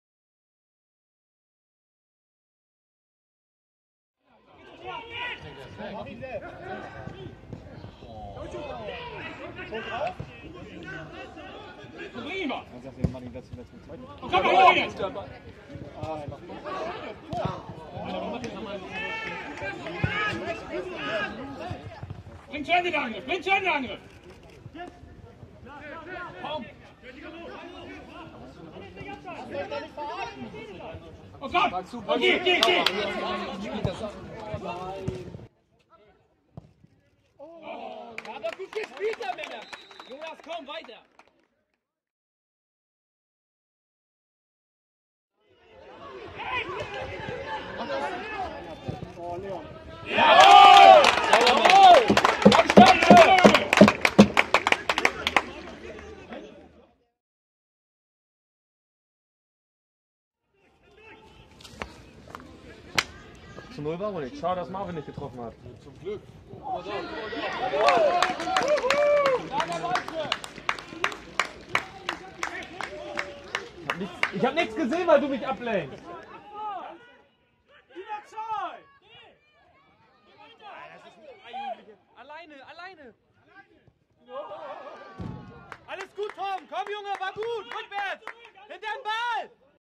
Highlights 29.08.2020 INTER TÜRKSPOR KIEL vs. TSV Klausdorf
0:1 Fey, Leon Luca (27.)
1:1 Jashari, Alban (29.)
1:2 Vetter, Bennet (46.)